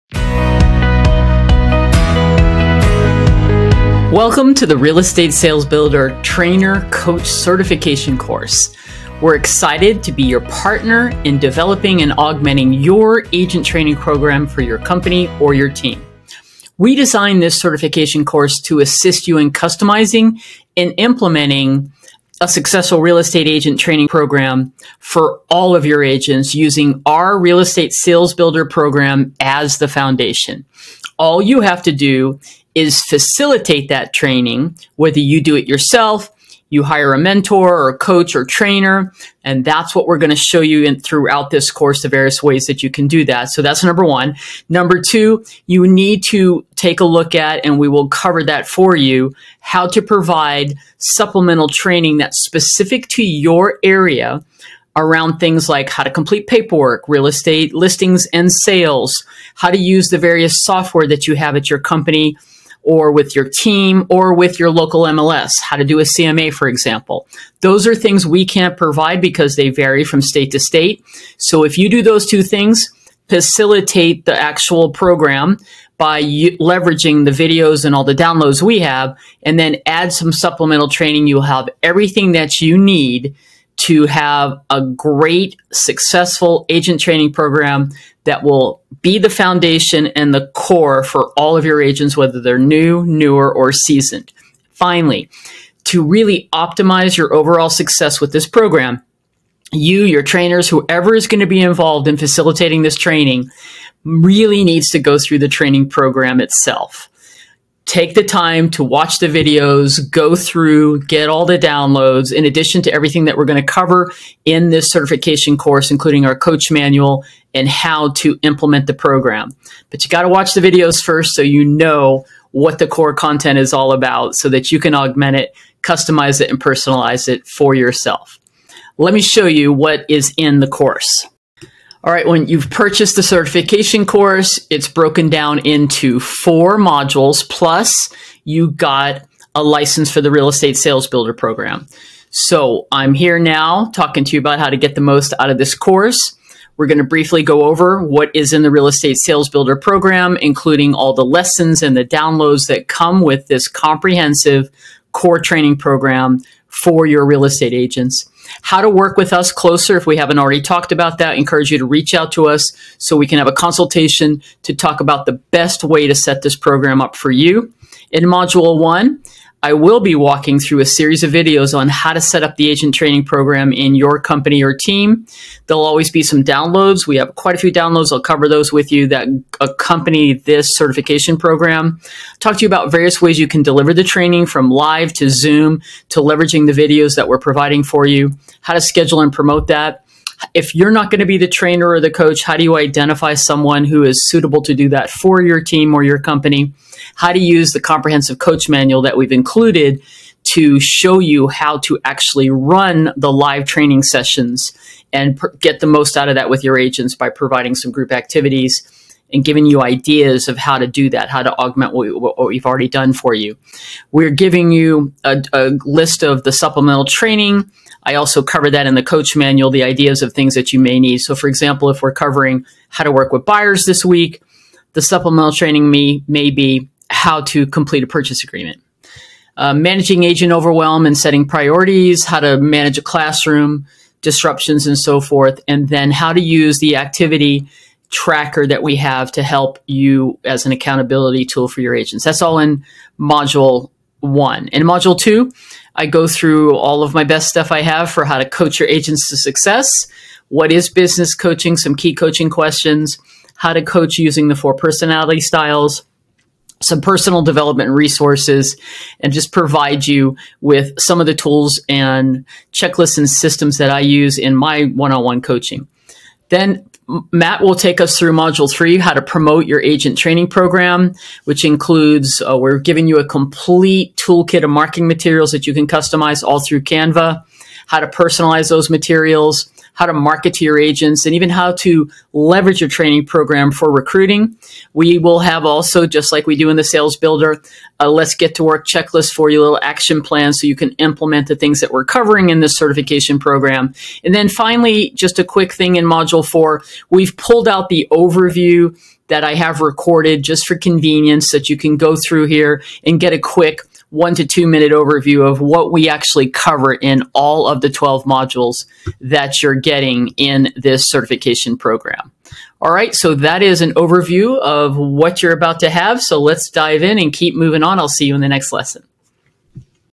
A Turnkey Agent Training Program with a Complete Coach and Mentor Certification Plan!
A Turnkey Agent Training Program with a Complete Coach and Mentor Certification Plan!  Looking for a turnkey training program?  We've got you covered!

This 4 Module, 53 lesson, certification course was created for coaches and mentors to fully understand and be able to deliver our Real Estate Sales Builder course to your agents.

As a BONUS, This course is accompanied by a single-user license for Real Estate Sales Builder.  Real Estate Sales Builder contains 12 training modules designed for both new and newer agents as well as any agent who wants to master the core fundamentals of a successful real estate business. The primary goal of this program is to show you how to consistently build your pipeline to generate more buyer and seller transactions to reach your individual business and career goals.

Welcome & Course Overview
Module 1: The Fundamentals of Real Estate & Your Business Plan
Module 2: Build Your Database & Get Referrals
Module 3: Effective Open Houses
Module 4: Working with Buyers
Module 5: Personality & Communication Styles
Module 6: Working with Sellers
Module 7: Listing Presentation & Marketing
Module 8: Farming Strategies
Module 9: Social Media & Online Presence
Module 10: Online Lead Generation & Conversion
Module 11: Niche Marketing, BDA & Getting Client Reviews
Module 12: Business & Financial Fundamentals
Real Estate Sales Builder has over 119 videos and nearly 100 downloads to help you build that strong foundation step by step!

00:00 Intro
00:16 Who was this designed for and how to get the most out of the course
02:09  An overview of the Real Estate Sales Builder Certification course
05:14 Wrap

🤫 Thinking of getting your real estate license?